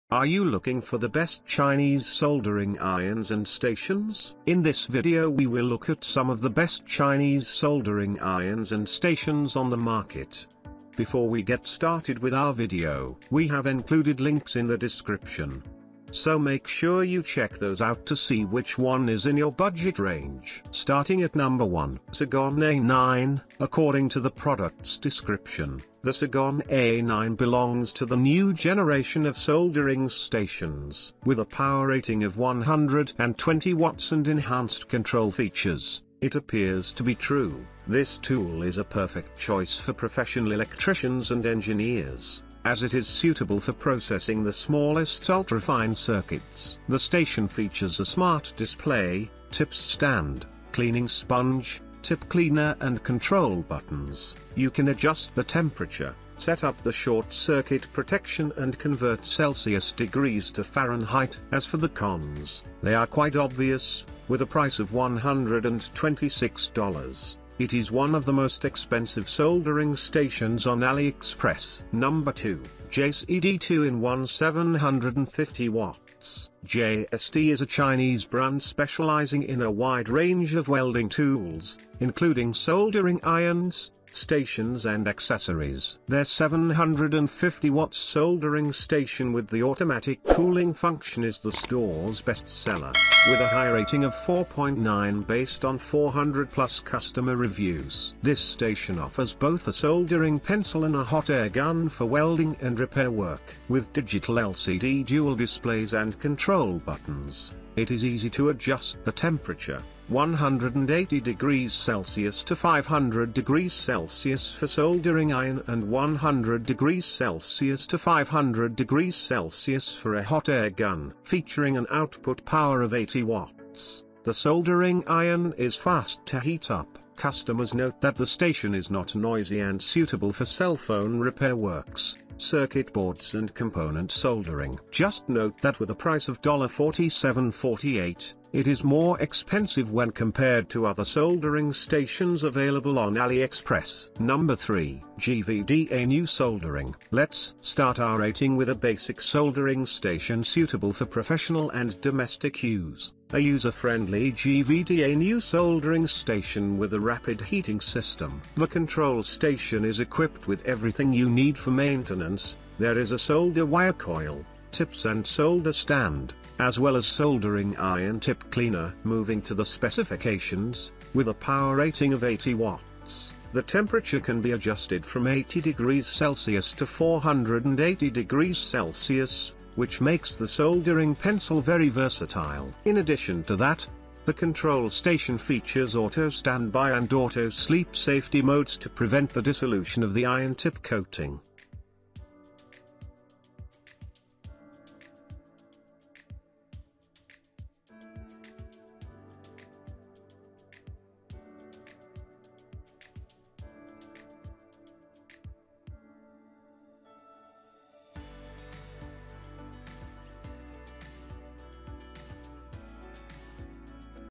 ✅Best Soldering Irons and Stations on AliExpress 2025
✅Best Chinese Soldering Irons and Stations on AliExpress 2025
1. SUGON-A9.

2. JCD 2 IN 1 750W.

3. GVDA NEW SOLDERING.

Hey guys in this video we are going to be checking out the Best Chinese Soldering Irons and Stations You Can Buy Right Now. We made This List Based On Our Personal Opinion and hours of research & we have listed them based on the type of features and Price. We have Included options for every type of user so whether you are looking for the best budget.
In this video, we listed the Best Short Throw Projectors.